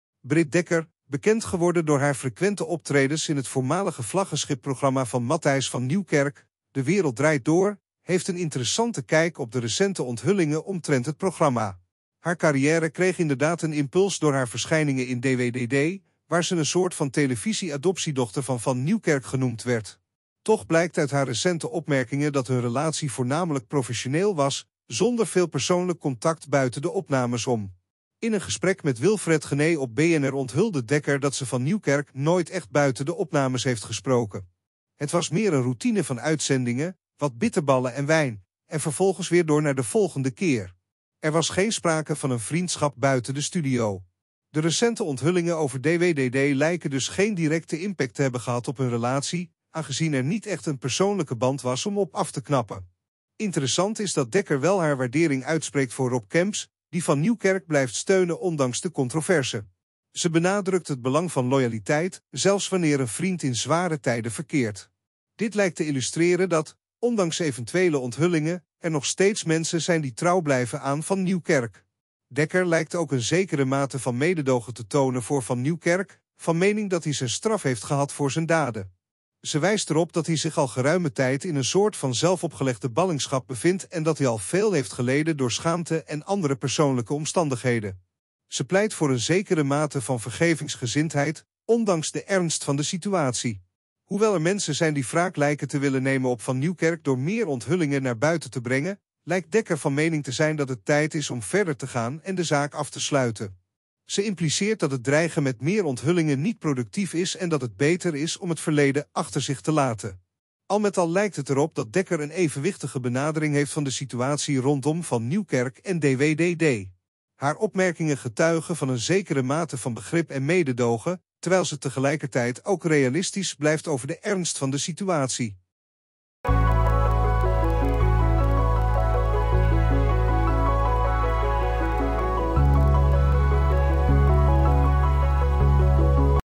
Britt Dekker afgeknapt op Matthijs van Nieuwkerk? ‘Geen contact’
Beschouw het als een kijkje achter de schermen: Britt Dekker deelt haar perspectief op de recente onthullingen over De Wereld Draait Door en Matthijs van Nieuwkerk. Ontdek hoe ze loyaliteit en vergevingsgezindheid balanceert in deze intrigerende discussie.